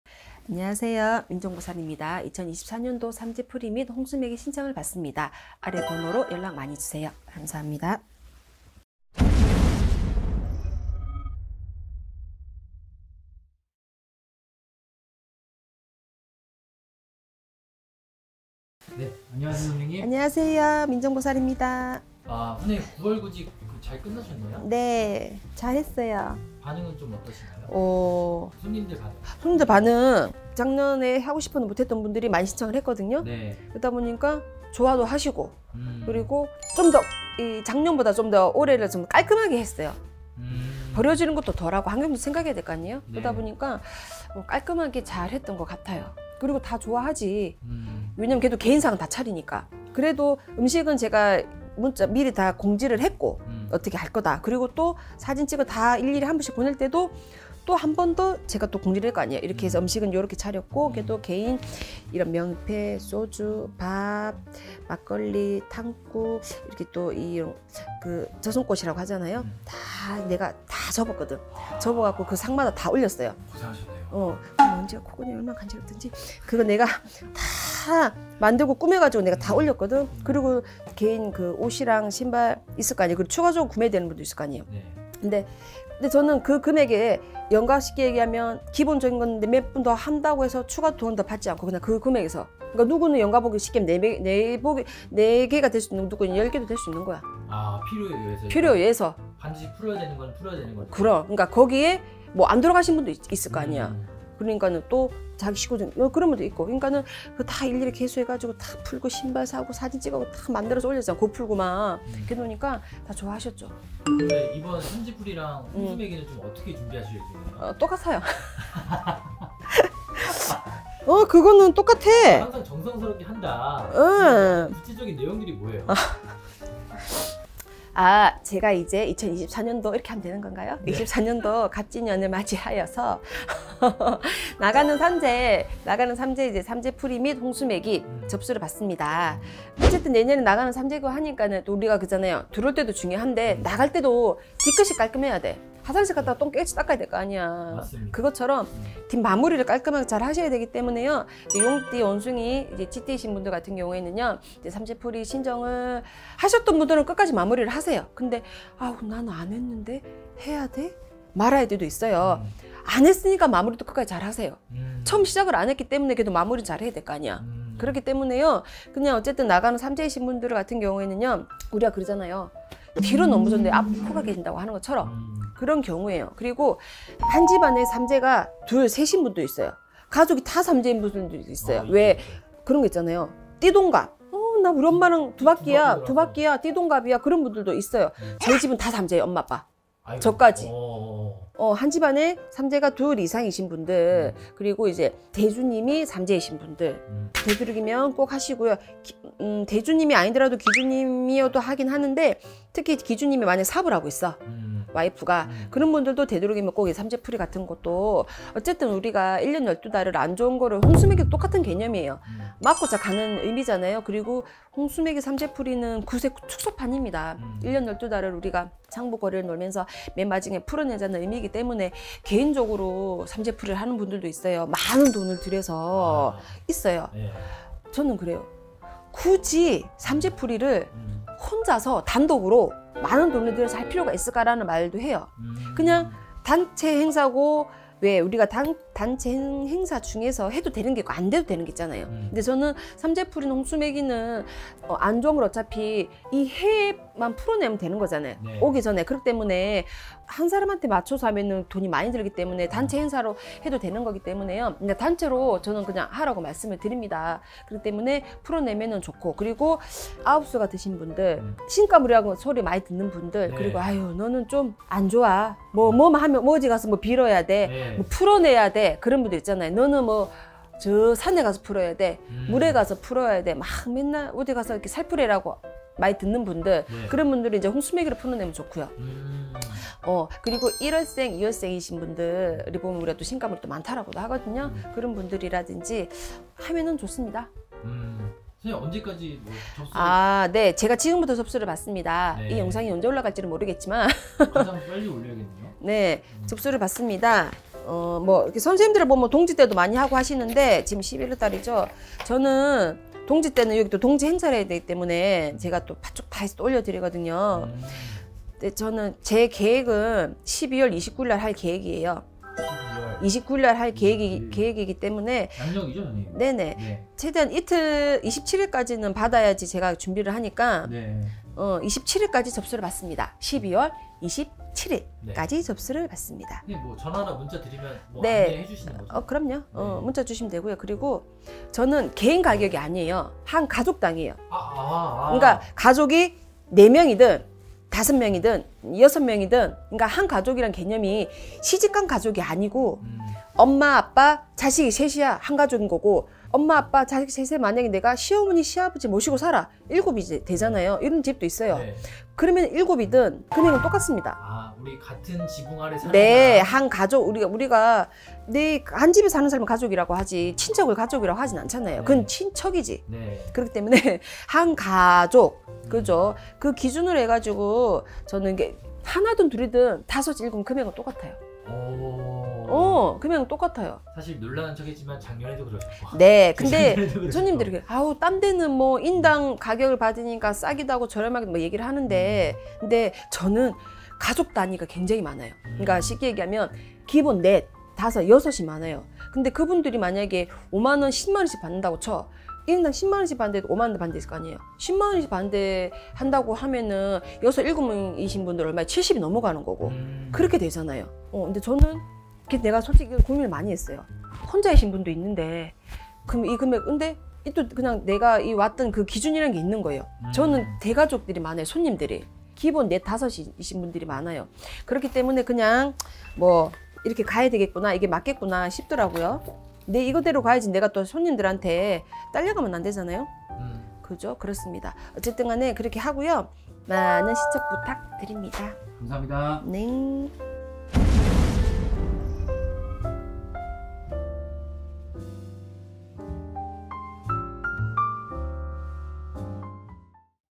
XX삼재인 분들 그냥 넘어가면 큰일이 닥친다?! 우리가 삼재풀이와 홍수맥이를 반드시 해야하는 이유!
★민정보살

민정보살 선생님은 덕분TV 소속 선생님이십니다

※민정보살 선생님 지난 영상